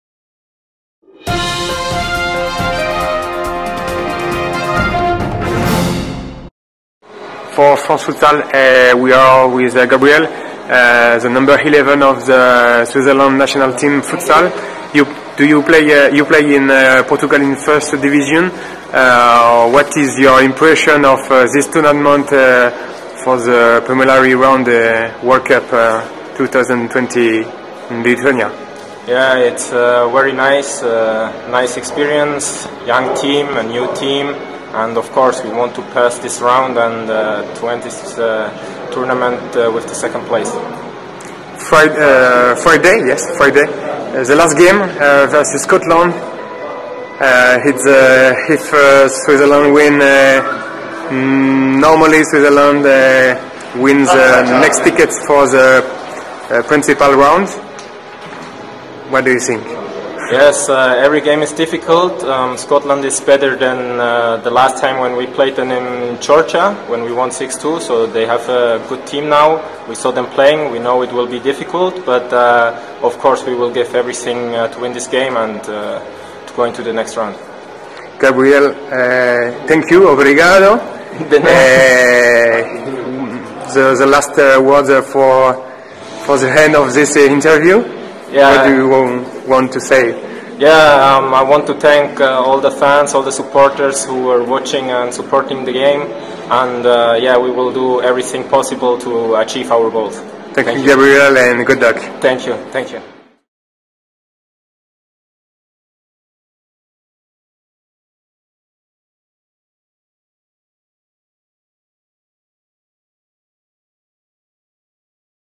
INTERVIEW DE GABRIEL BUCKSON
Interview de Gabriel Buckson après la victoire de suisse 4-1 face à l'Ecosse.